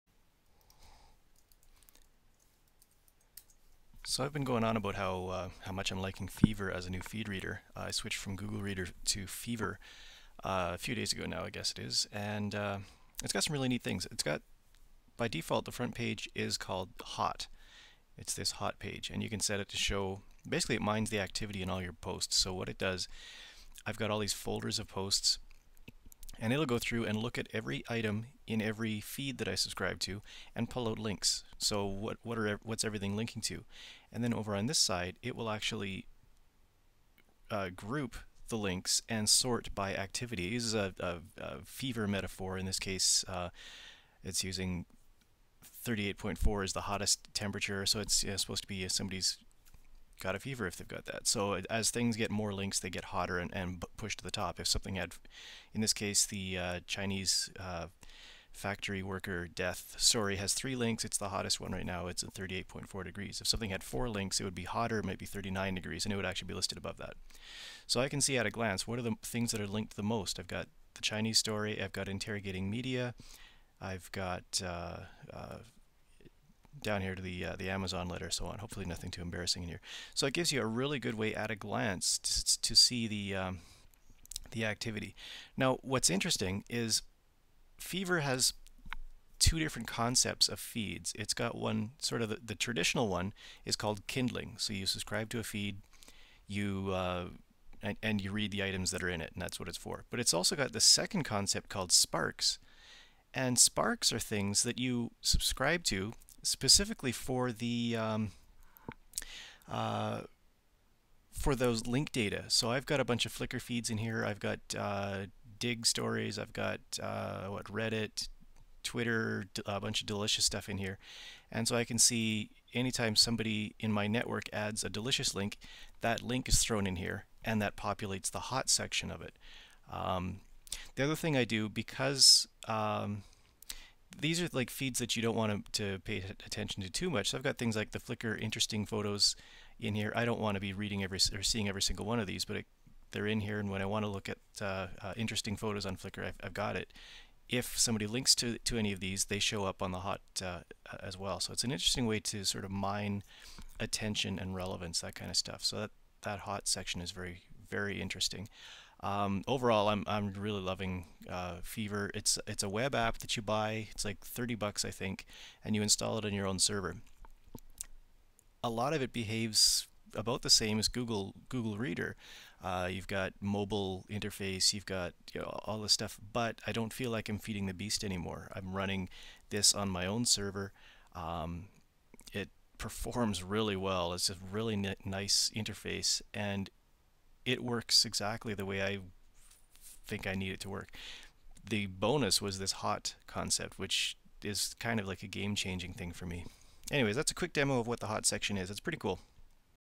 Fever˚ Hot demo
a quick demo showing how the Fever˚ feed reader works, with the awesome Hot feature that mines feed items to find most-linked URLS.